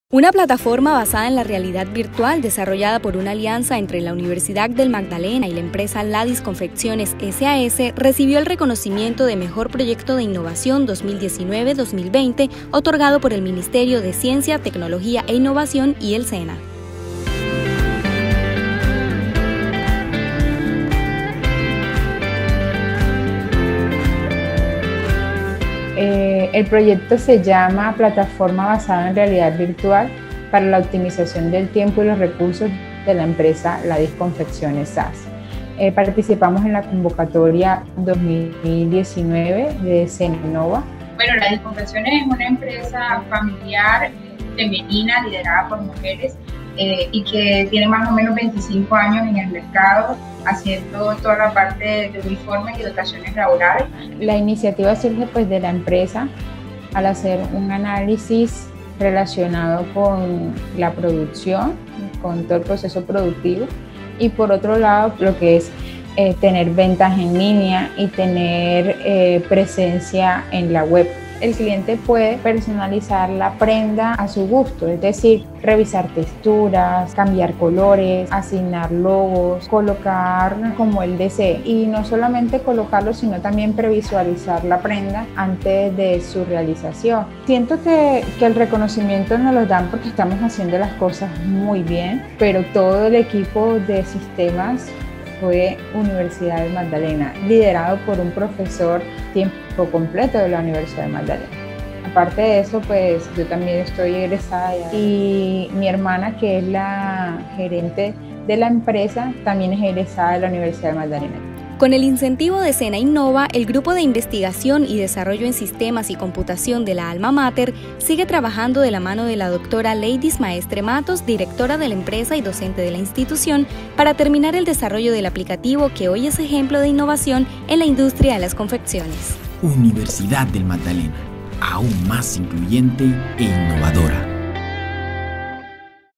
Ingenio de UNIMAGDALENA, reconocido como mejor proyecto de SENA Innova
El Ministerio de Ciencia, Tecnología e Innovación y el SENA premiaron como Mejor Proyecto de Innovación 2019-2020 a una plataforma desarrollada por el Grupo de Investigación y Desarrollo en Sistemas y Computación de la Universidad del Magdalena y la empresa Ladys Confecciones S.A.S.

La Alma Mater y la compañía dirigida por la doctora Leydis Maestre Matos, docente de las Facultades de Ciencias Empresariales e Ingeniería, crearon una página web en realidad virtual para la personalización de prendas de manera remota, por lo que hoy son ejemplo en la industria textil.

Con el incentivo de Sena Innova, se concluirá el desarrollo del aplicativo para la optimización del tiempo y los recursos de la empresa.